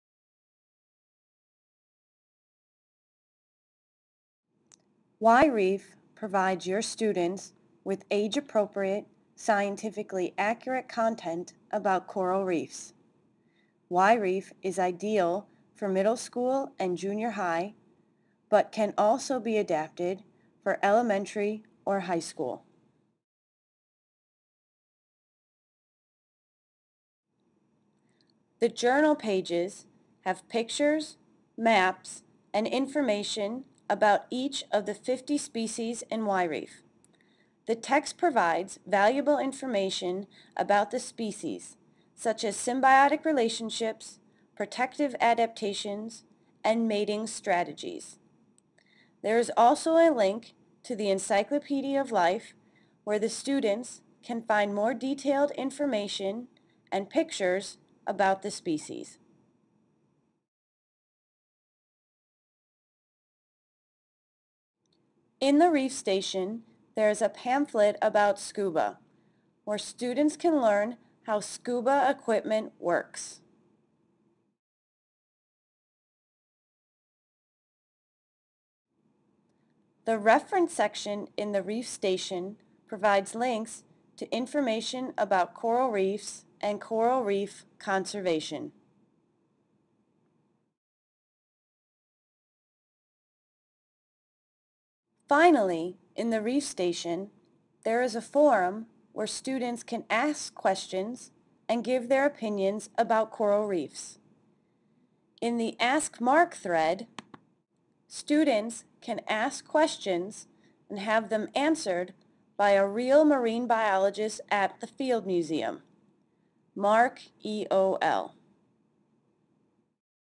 Research Tools In WhyReef
WhyReef provides your students with age-appropriate, scientifically accurate content about coral reefs.  WhyReef is ideal for middle school and junior high, but can also be adapted for elementary and high school.